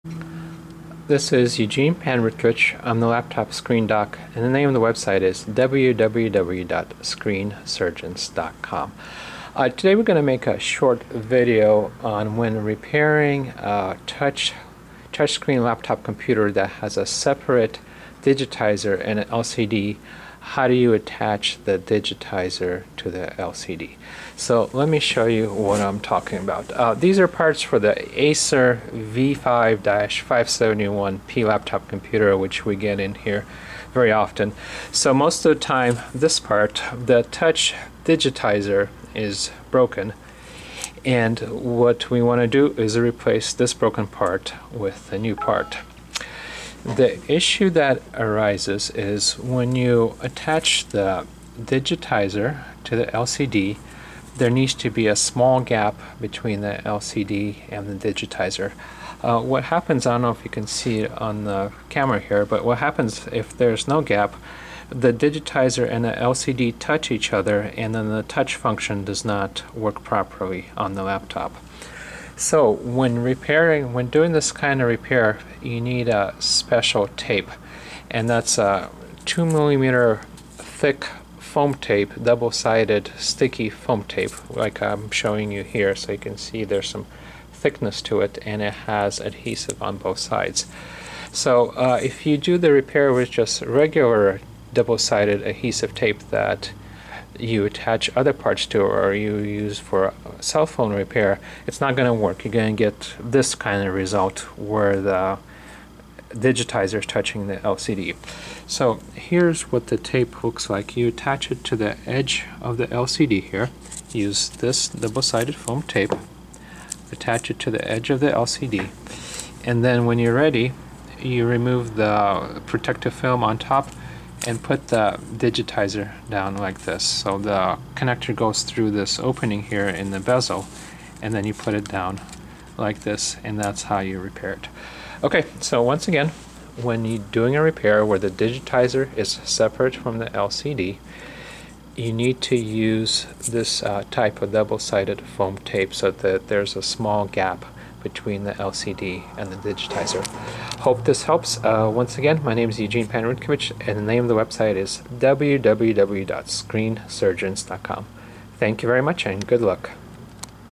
How to Attach the Digitizer to the LCD When Repairing a Touchscreen Laptop
When repairing a touchscreen laptop with a broken digitizer, the new digitizer needs to have a small gap between it and the LCD. This video